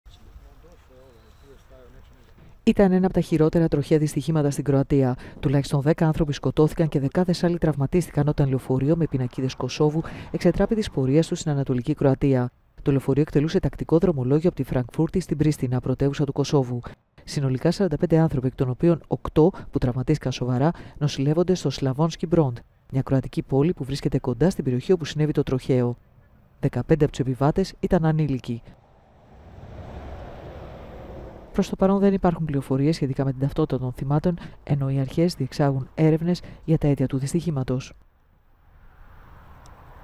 Πολύνεκρο τροχαίο στην Κροατία-Στο πένθος βυθίστηκε το Κόσοβο
Οι αρχές διεξάγουν έρευνες για τα αίτια του δυστυχήματος.